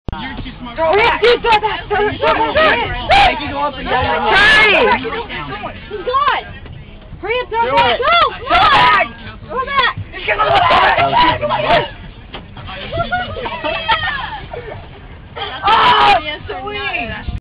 giant rubberband tossing.
so we found this giant rubber band that's longer than the bus when stretched out when we were at state fair. and soo... we tossed it back and forth between the busses.  idk why.